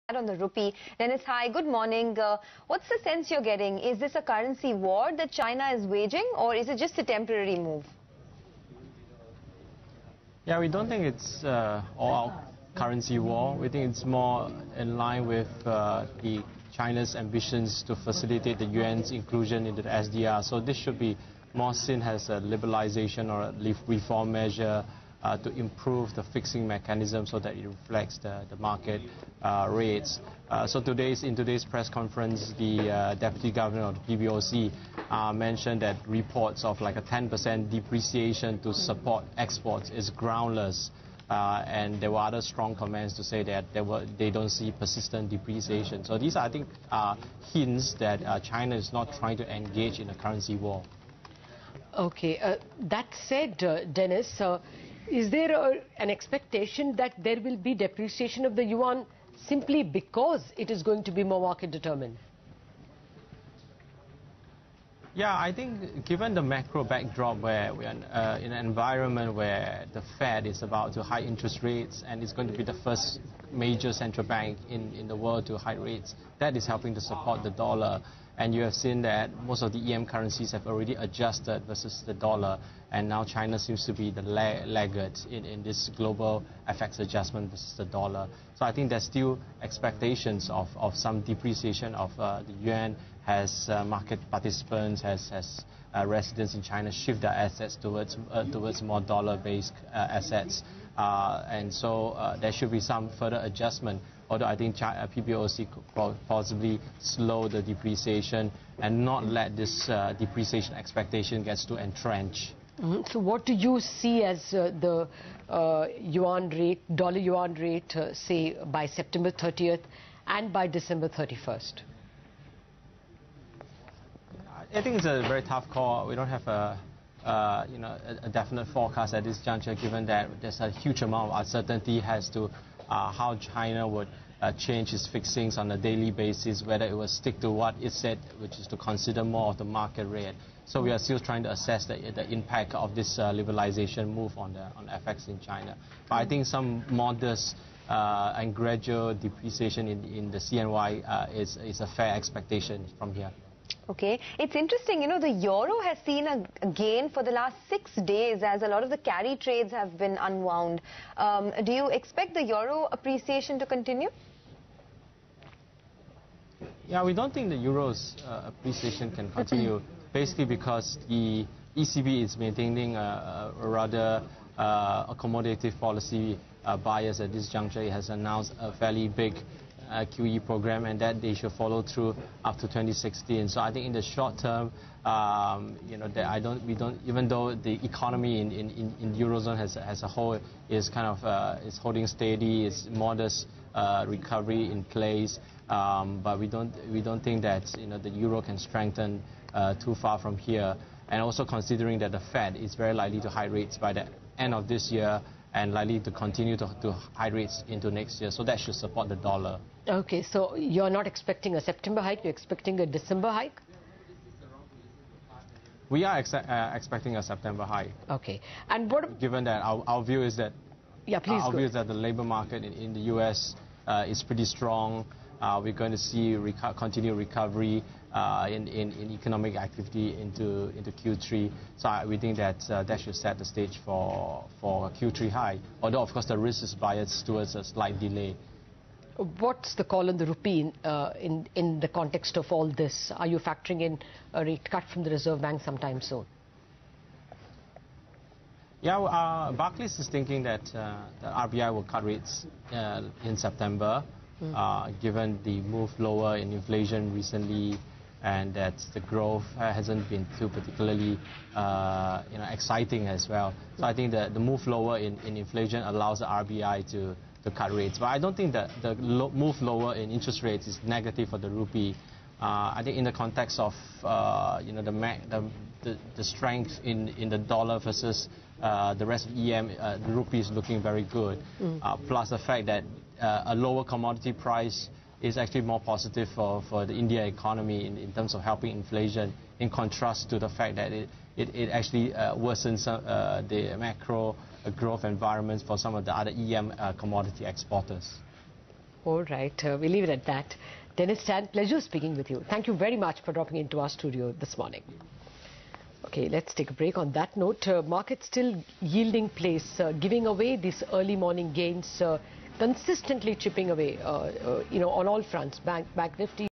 No Currency War By China :Barclays - August 13
Dennis Tan the FX Strategist at Barclays talks about the impact of Yuan devaluation on Indian market and currency